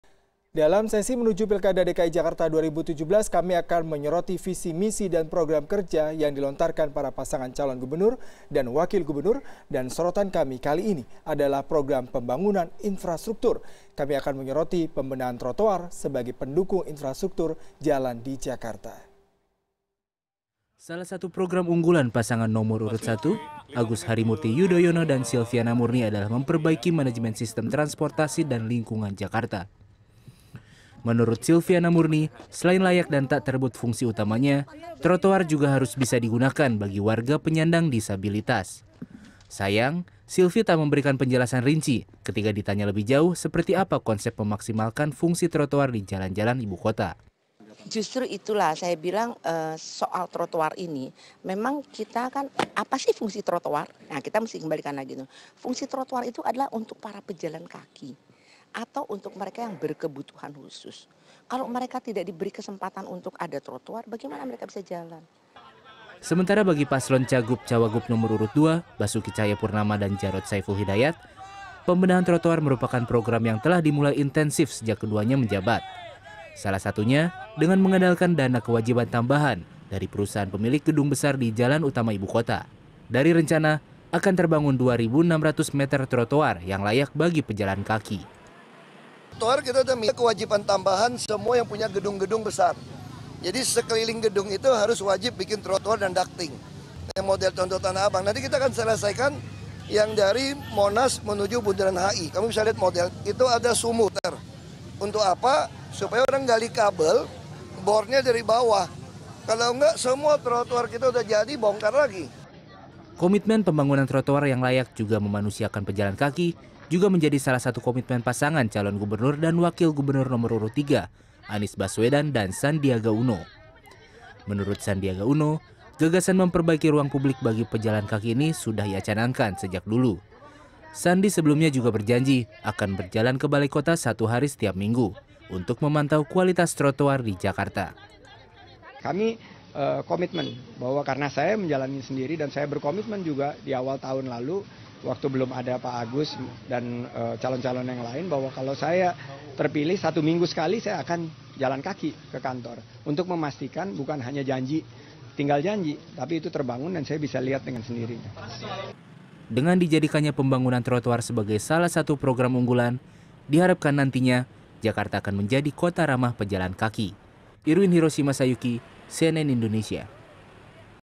Pembenahan Trotoar Jakarta di Mata Cagub-Cawagub
Dalam sesi menuju Pilkada DKI Jakarta 2017, kami akan menyoroti, visi misi dan program kerja, yang dilontarkan, para pasangan Calon Gubernur, dan Wakil Gubernur. Dan sorotan kami kali ini, adalah program pembangunan infrastruktur.

Kami akan menyoroti pembenahan trotoar, sebagai pendukung infrastruktur jalan di Jakarta. Berikut penuturan Cagub-Cawagub DKI.